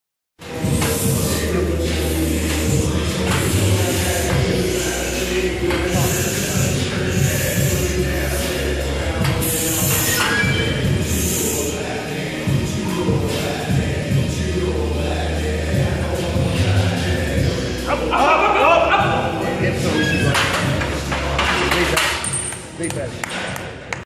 Chris Fisher squats 250kg PTC HQ
10kg gym PB and 50kg over his comp best. Chris will go around 680kg at GPC Vic States on the 22nd of Feb